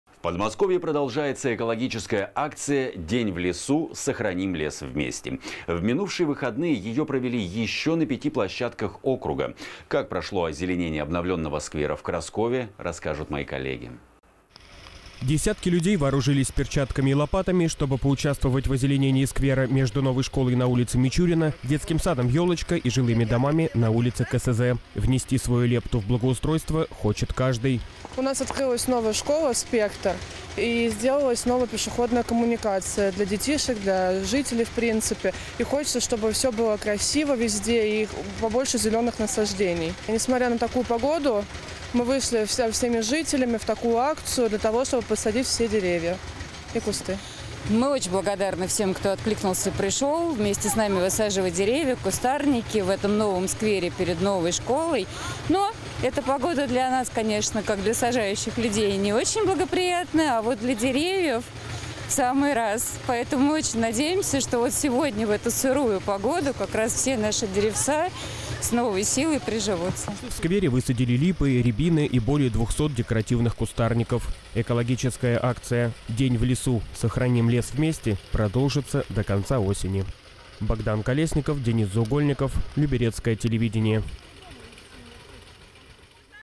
Новый облик зоны для тихого отдыха
В Подмосковье продолжается экологическая акция «День в лесу. Сохраним лес вместе». В минувшие выходные её провели ещё на пяти площадках округа. Как прошло озеленение обновлённого сквера в Краскове, расскажут мои коллеги.

Десятки людей вооружились перчатками и лопатами, чтобы поучаствовать в озеленении сквера между новой школой на улице Мичурина, детским садом «Ёлочка» и жилыми домами на улице КСЗ. Внести свою лепту в благоустройство хочет каждый.

Анастасия Мусина, жительница г.о. Люберцы: «У нас открылась новая школа «Спектр» и сделалась новая пешеходная коммуникация для детишек, для жителей в принципе. И хочется, чтобы всё было красиво везде и побольше зелёных насаждений. И, несмотря на такую погоду, мы вышли со всеми жителями на такую акцию для того, чтобы посадить все деревья и кусты».

Наталья Романцова, заместитель главы г.о. Люберцы: «Мы очень благодарны всем, кто откликнулся и пришёл вместе с нами высаживать деревья, кустарники в этом новом сквере перед новой школой. Но эта погода для нас, конечно, как для сажающих людей не очень благоприятная, а вот для деревьев в самый раз, поэтому мы очень надеемся, что сегодня в эту сырую погоду как раз все наши деревца с новой силой приживутся».

В сквере высадили липы, рябины и более 200 декоративных кустарников. Экологическая акция «День в лесу. Сохраним лес вместе» продолжится до конца осени.